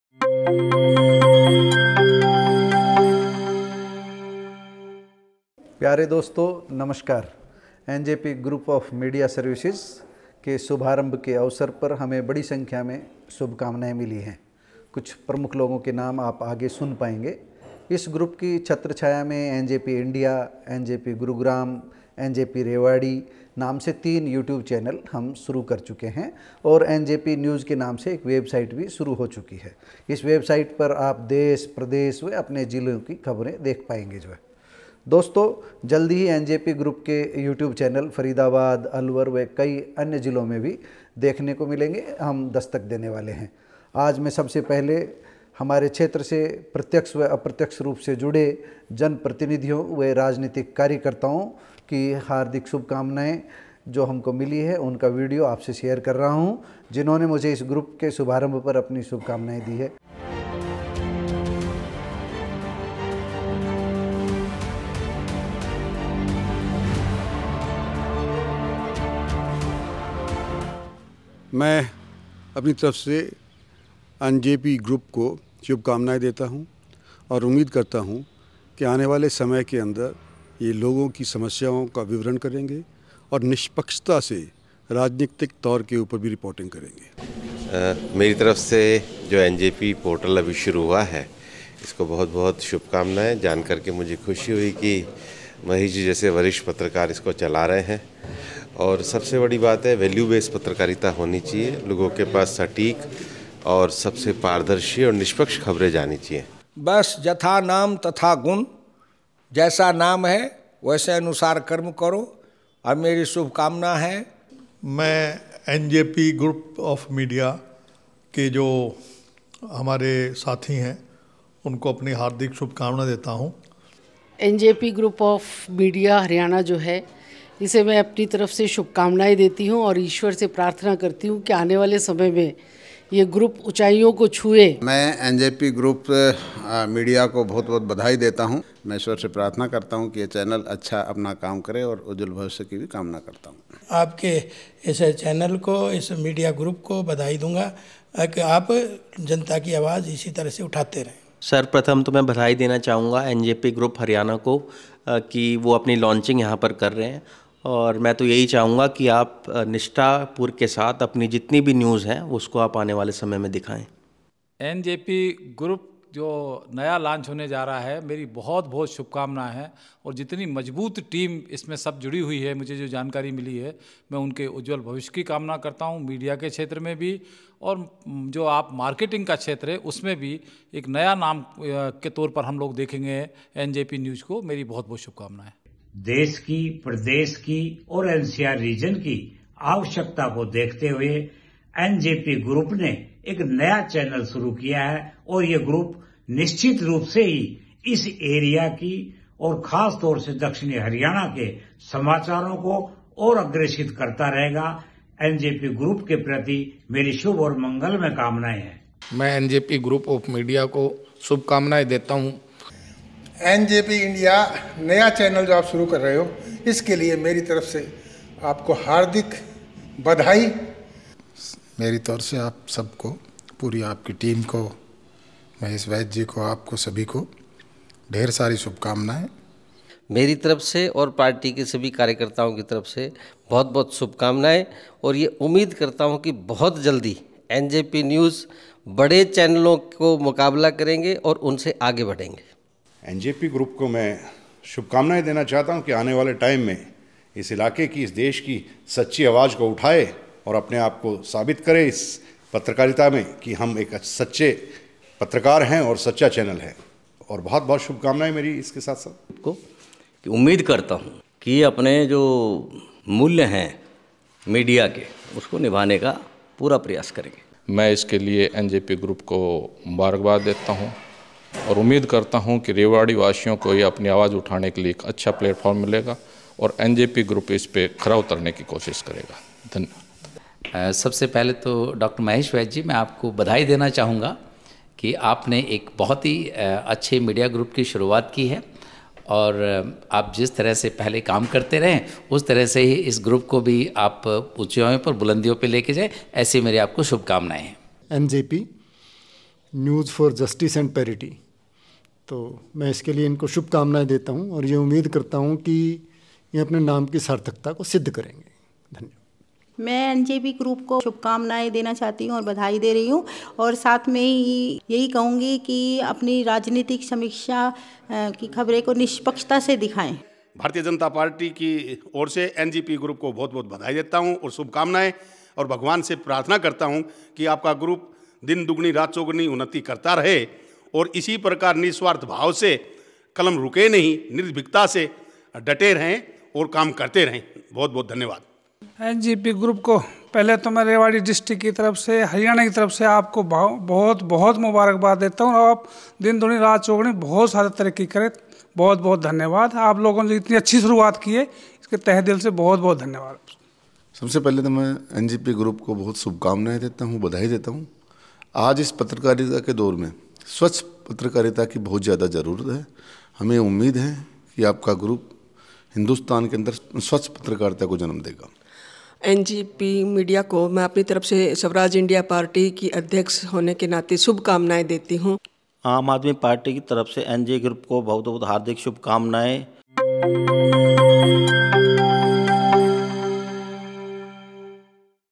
THE LAUNCHING OF NJP INDIA
NJP India एक यूटयूब चैनल है। एनजेपी मतलब न्यूज फार जस्टिस एंड पैरिटी। जस्टिस यानी न्याय और पैरिटी मतलब समानता। जहां न्याय वहीं समानता, जहां समानता वहीं न्याय। खबरों व मुद़दों के चयन और उनकी समीक्षा का हमारा आधार यही रहेगा। देश और राज्य स्तर की खबरों के लिए NJP INDIA सर्च करें और अपने शहर, अपने गांव, अपने आसपास और जिला स्तर की खबरों के लिए NJP REWARI, NJP GURGAON, NJP FARIDABAD या NJP ALWAR आदि सर्च करे। सबसे पहले हम एनजेपी इंडिया और एनजेपी रेवाड़ी व गुरुग्राम से शुरूआत करने जा रहे हैं। यदि आपका साथ व प्यार मिला तो जल्द ही आपके शहर में भी शुरूआत करेंगे। आपको नीचे डिस्क्रेप्शन में सभी लिंक मिल जाएंगे। आपका सहयोग ही हमारी प्रेरणा बनेगा।